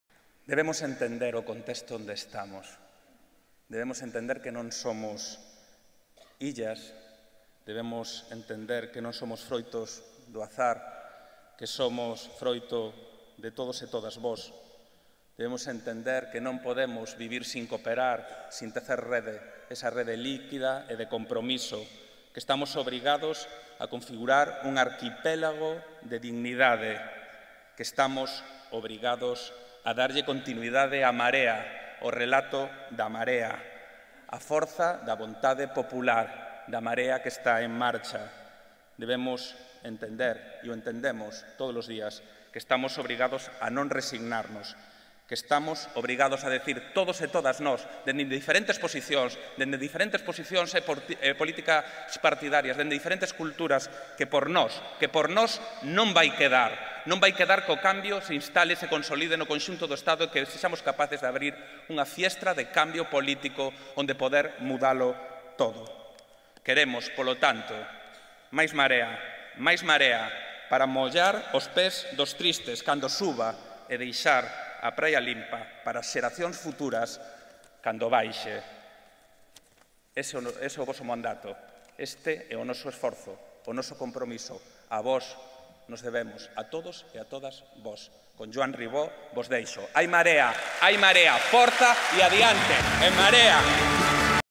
Martiño Noriega: "Estamos obrigados a cooperar"
Martiño Noriega, no acto das CidadesRebeldes organizado pola Marea Atlántica tamén falou para todas nós: "Estamos obrigados a darlle continuidade ao relato da Marea, estamos obrigados a non resignarnos, estamos obrigados a configurar un arquipélago de dignidade, estamos obrigados a cooperar"